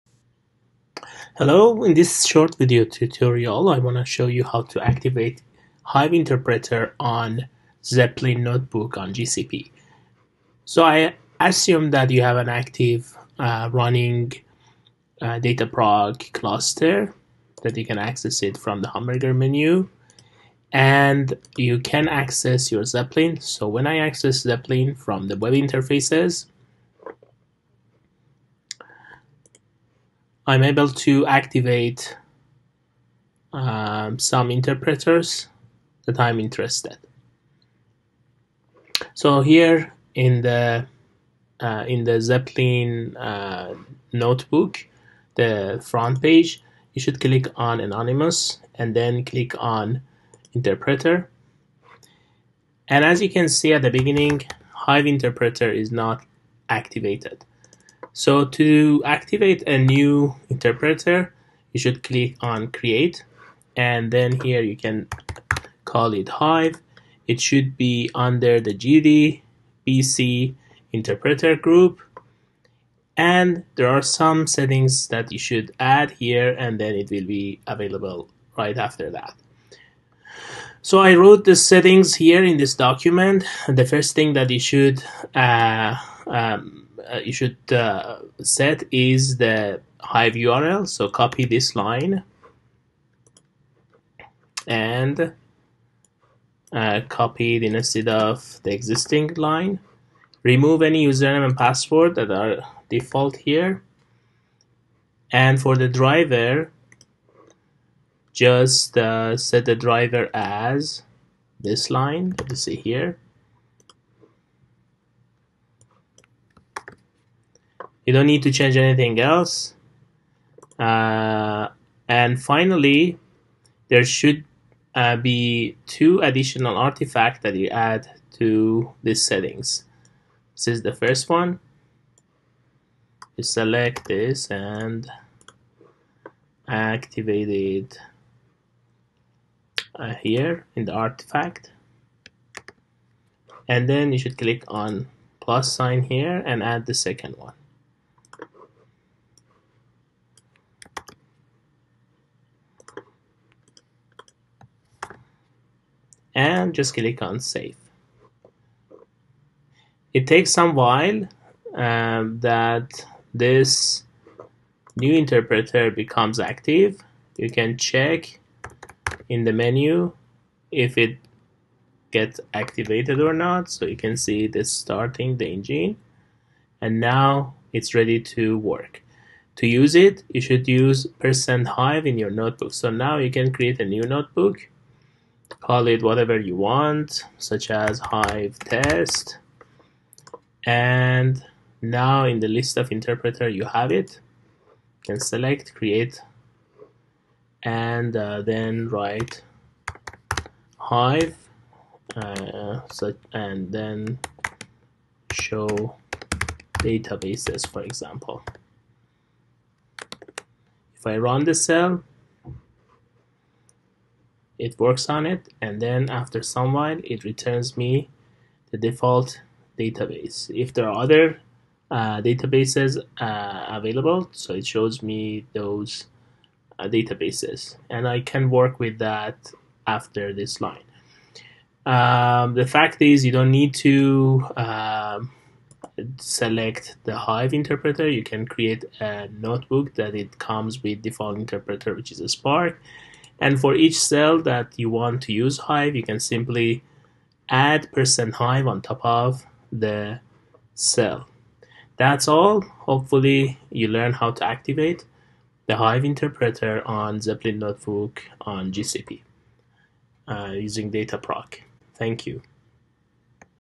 How to activate hive interpreter for Apache Zeppelin on Dataproc on GCP? (Code in description)
1. Set hive.url as
jdbc:hive2://localhost:10000

2. Remove username and password (keep them empty)

3. Set hive.driver property as:
org.apache.hive.jdbc.HiveDriver

4. Adding two artifact under dependencies tab:
org.apache.hive:hive-jdbc:0.14.0
org.apache.hadoop:hadoop-common:2.6.0